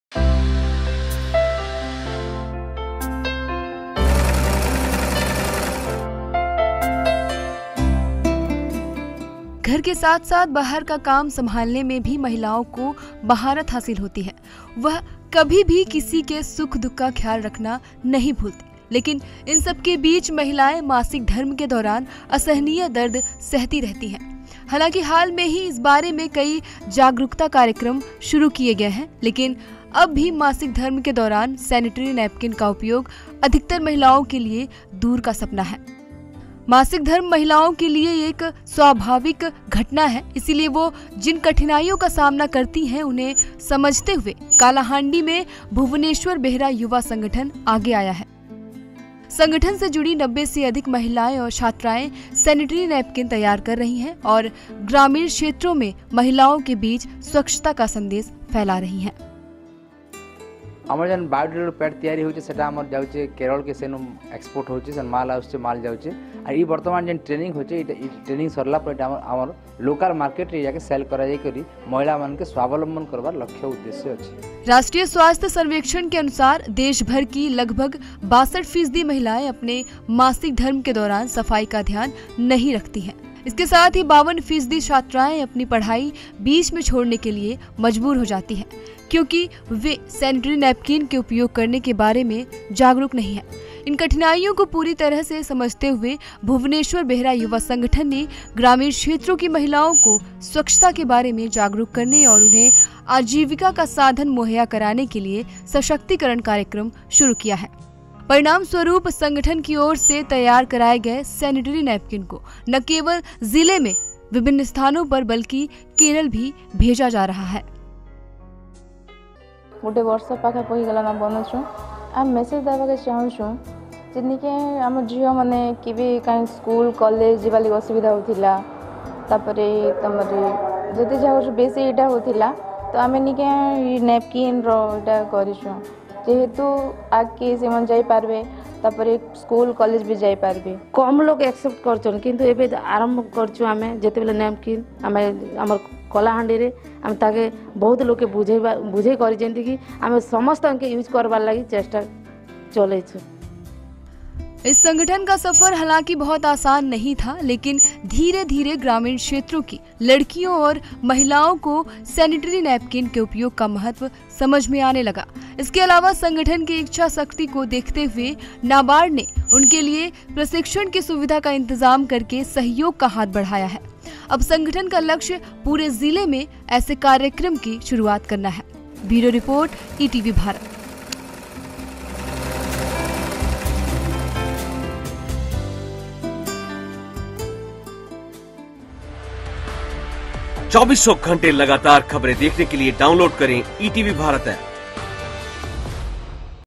कालाहांडी की पैड महिलाएँ | Pad Women Of Kalahandi | ETV Bharat Delhi
ओडिशा के कालाहांडी में भुवनेश्वर बेहरा युवा संगठन में महिलाओं की मासिक धर्म से जुड़ी समस्या के निवारण के लिए सराहनीय कदम उठाया है. गाँव की कुल 90 महिलाओं के साथ भुवनेश्वर में स्वच्छता का संदेश फैलाने के लिए ये महिलायें "सैनिटरी नैपकिन" तैयार करती हैं। राष्ट्रीय स्वास्थ्य सर्वेक्षण के अनुसार लगभग 52 प्रतिशत लड़कियां पढाई को बीच में छोड़ देती हैं क्यूंकि वो "सैनिटरी नैपकिन" से अनजान हैं। इन महिलाओं का मकसद अपने सभी उत्पादों को स्थानीय बाजार में बेचकर खुद को आत्मनिर्भर बनाना है। आइये देखते हैं कालाहांडी की पैड महिलाओं की कहानी उन्ही की जुबानी।